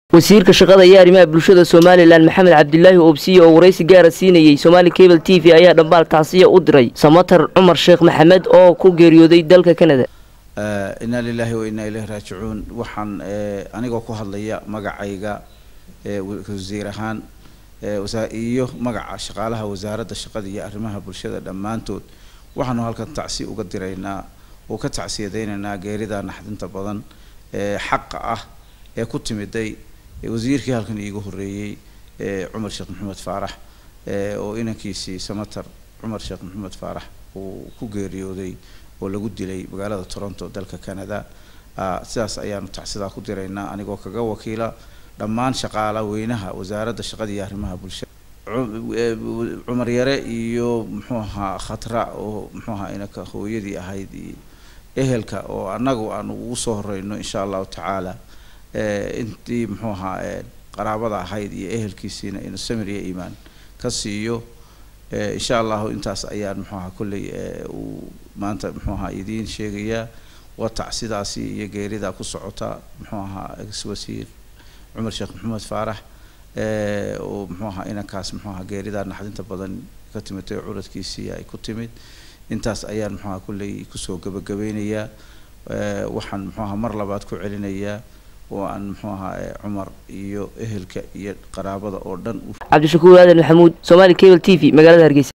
Daawo Wasiirka Shaqada Iyo Arimaha Bulshada Somaliland Oo Tacsi Udiray Wasiirkiihore
Weriye Cabdi Jabaan